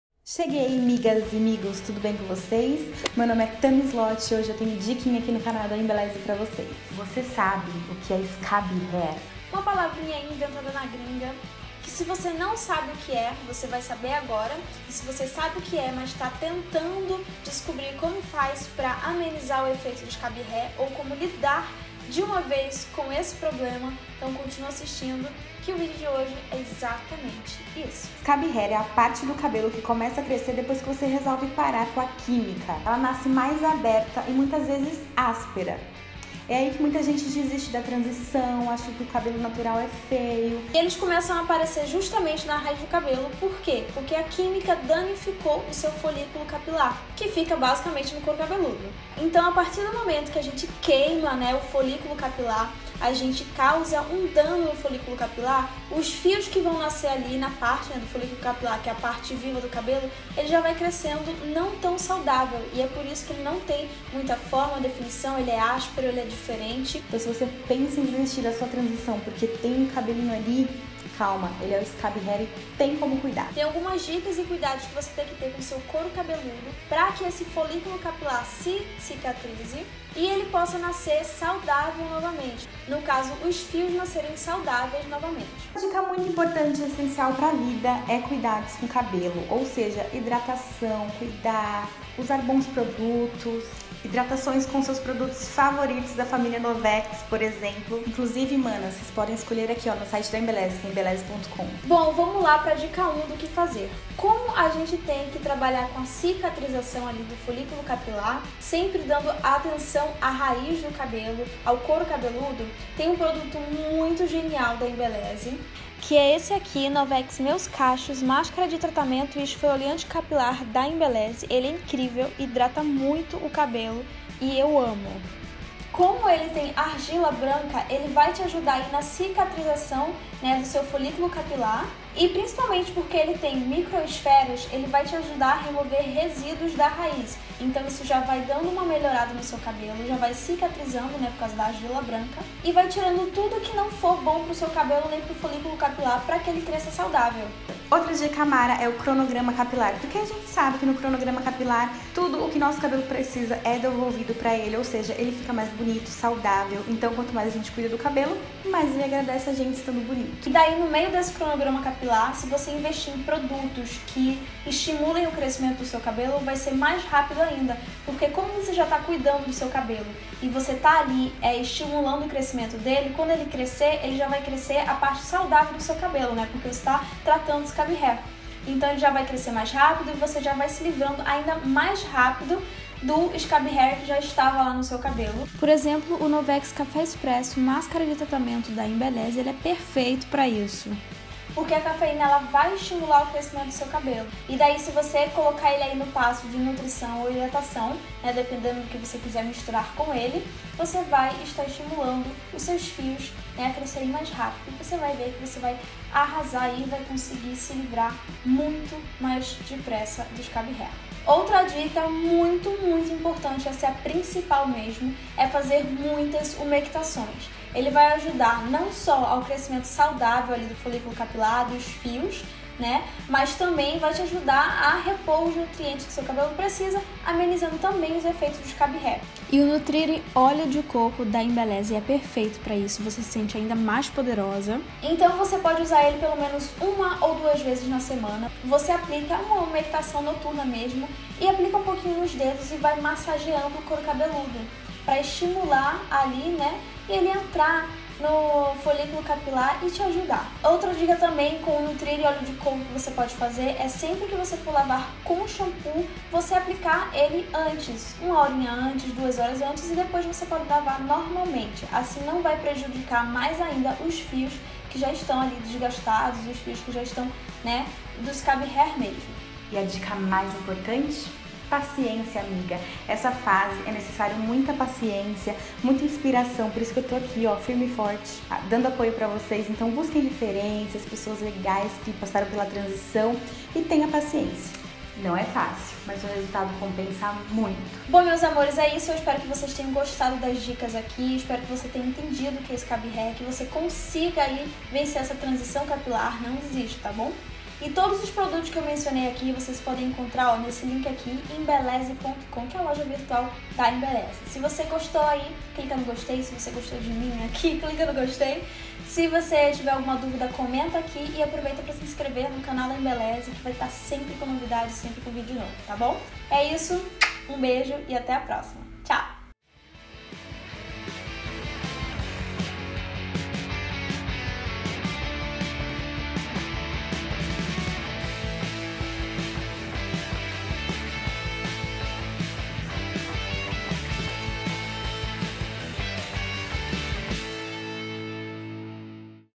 Como tratar o Scab Hair com Tarcyla Guiotto e Tammy Sloty
Scab Hair. O que é? Tem jeito? Tem como tratar? Conheça as dicas das lindas Tarcyla e Tammy!
Se increva no canal da Embelleze, ative o sino para receber as notificações  e sempre compartilhe os bons vídeos!